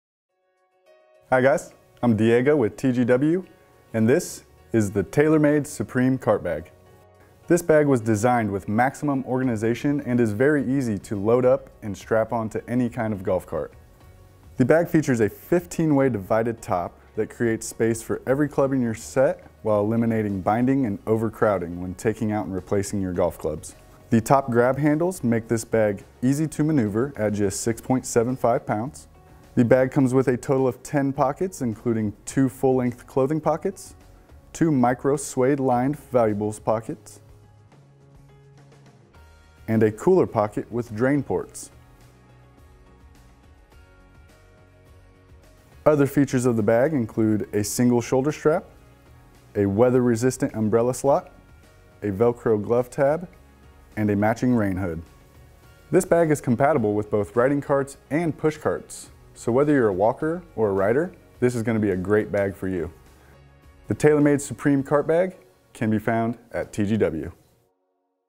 TaylorMade 2017 Supreme Cart Bag
The TaylorMade 2017 Supreme cart golf bag lives up to its name and loads up on storage and organization. 15 dividers create a spot for every golf club and eliminates crowding. A total of 10 pockets hold everything from drinks balls tees valuables and more so you can load it up and strap it to any cart. Crush resistant and durable this cart bag will keep you playing on round after round.